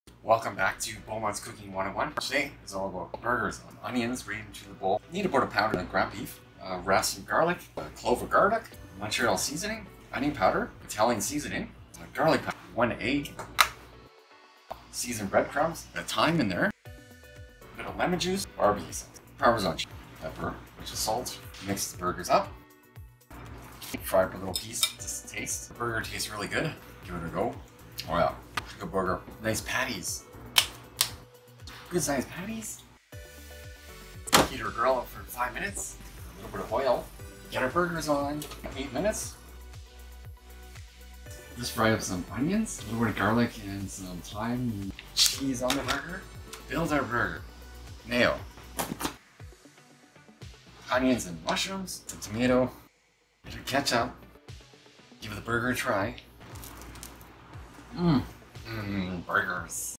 Homemade beef burgers Best simple burgers.
You will need -1 Lbs. of Ground Beef -Salt and Pepper to Taste -4-5 Button Mushrooms Sliced -2 Slices of Tomatoes -1 Tbsp of BBQ Sauce -Garlic Powder -Onion Powder -Italian Seasoning -Montreal Steak Spice -Seasoned Toasted Bread Crumbs -1 Egg -Squeeze of Lemon Juice -Parmesan Cheese For Saute Onions and Mushrooms -1 Onion Sliced -6-7 sliced Button Mushrooms -1 Tsp. of Thyme -2 Tsp. of Butter -1 Clove of Garlic finely Chopped
Enjoy making easy fun simple Burgers. Very Tasty. Add other veggies and lettuce is Great too be Creative with your homemade burgers. Enjoy and have fun.
18+ Shirtless and Nude Cooking.